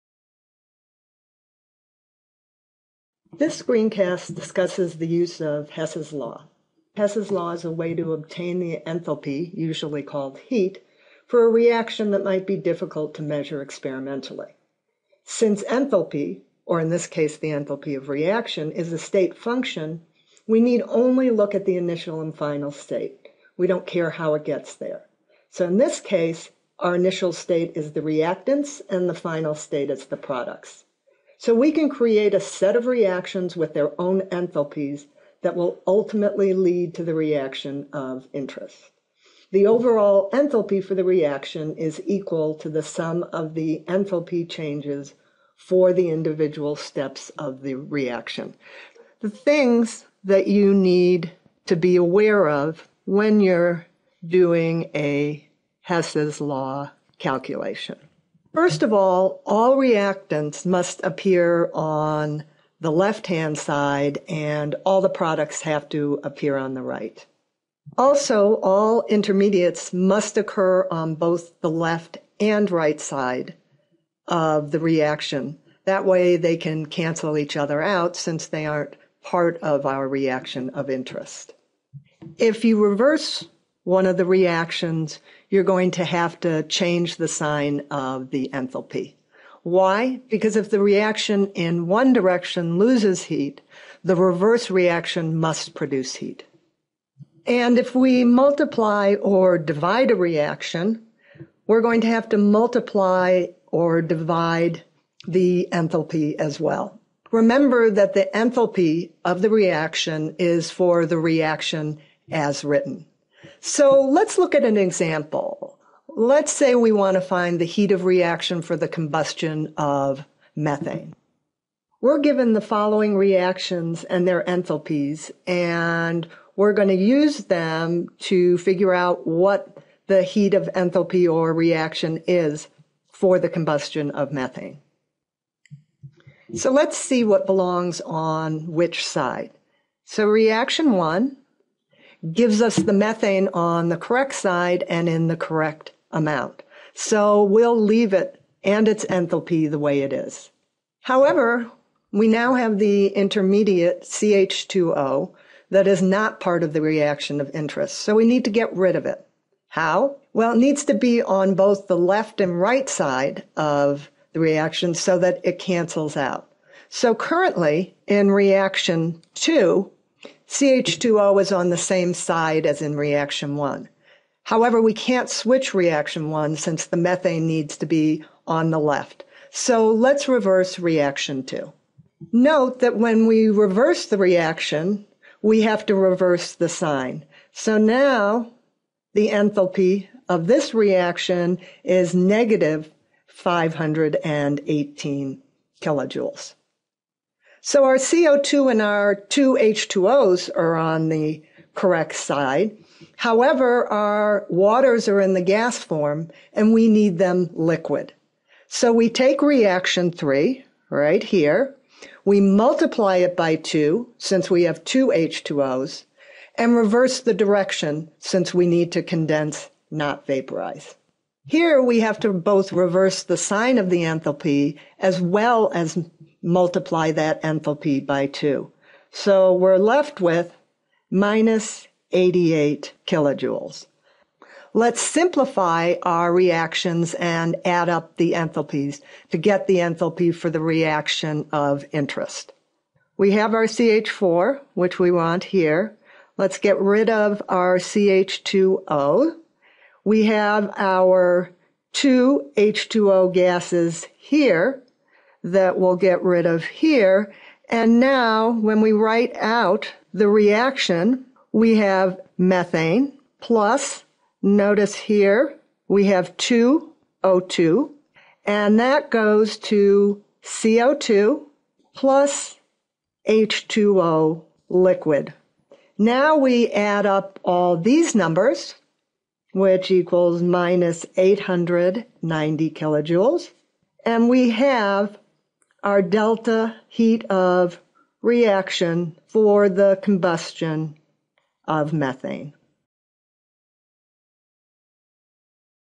Hess's Law
Organized by textbook Explains Hess's law and provides an example of how to use it to solve for the heat of reaction for an equation.  Made by faculty at the University of Colorado Boulder, Department of Chemical & Biological Engineering.